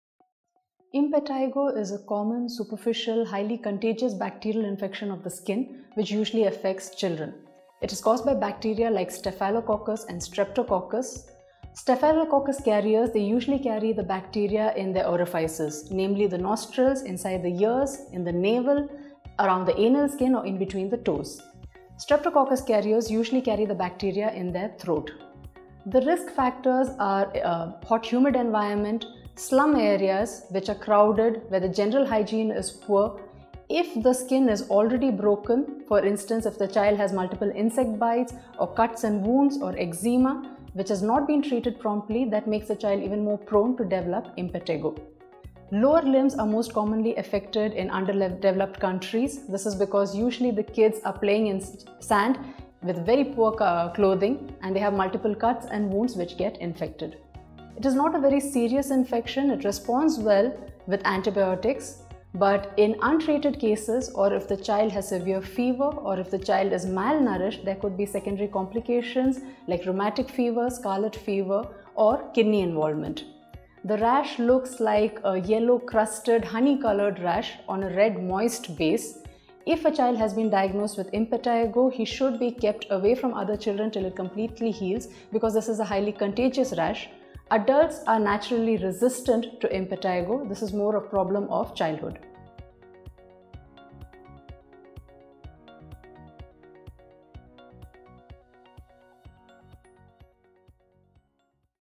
Impetigo Causes, Symptoms, Treatments
What do you need to know about impetigo? In this video, Dr.Sunaina familiarises us with the condition, its causes, symptoms, and treatment.